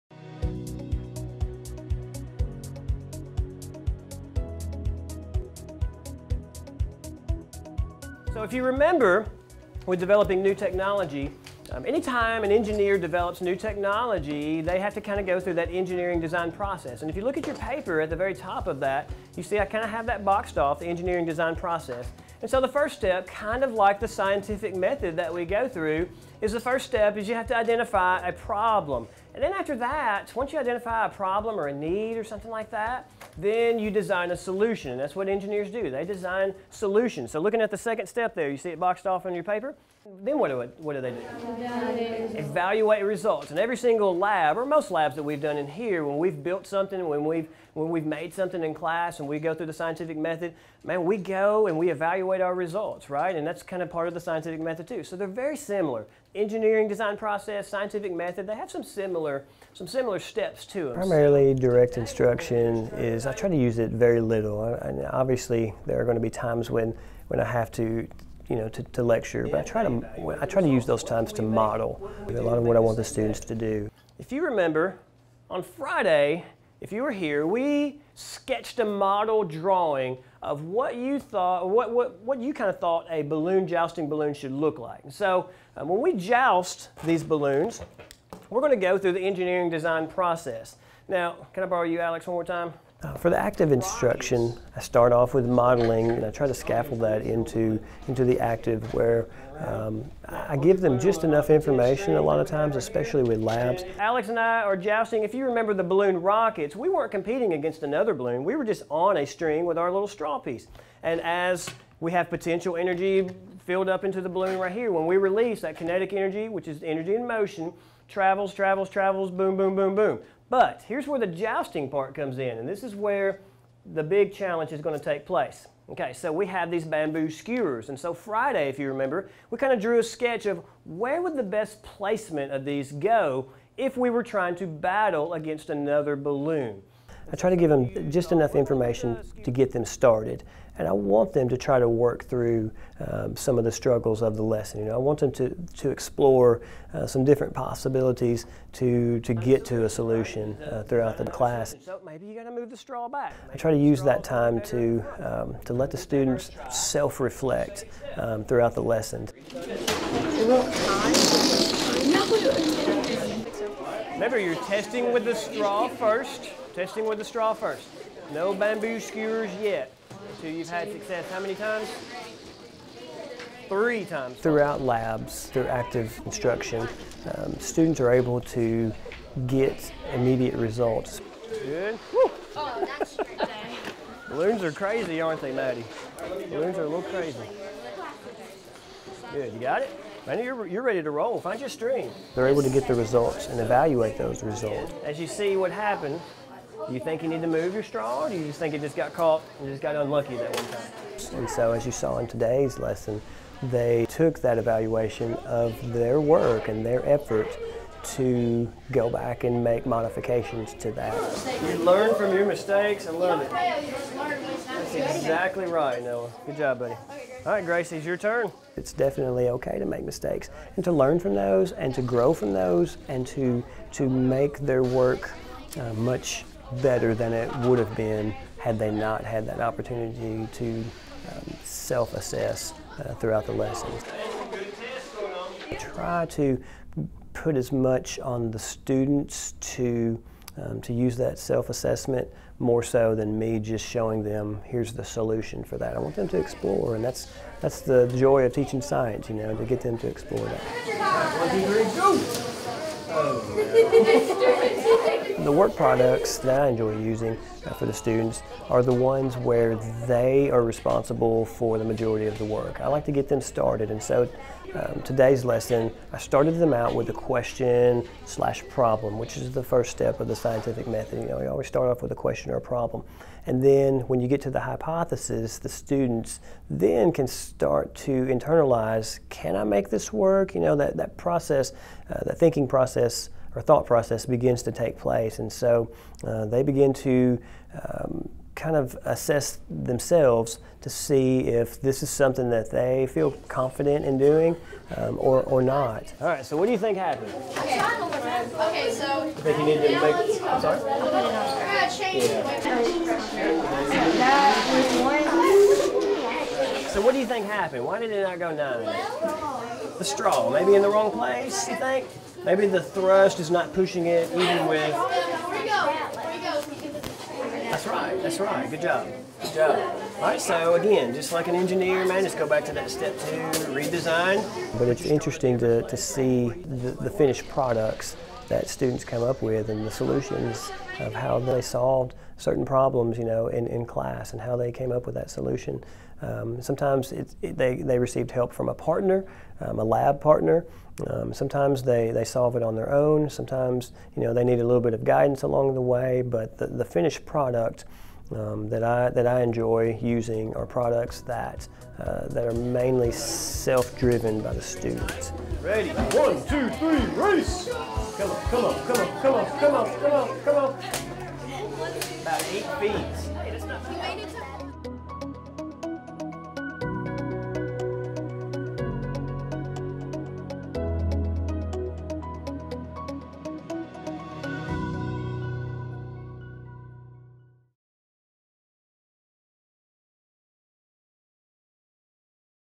Balanced Instruction (Secondary)
Video featuring teacher Mike French describing how he incorporates Balanced Instruction strategies in his sixth-grade science class. This video was developed for the Tennessee Department of Education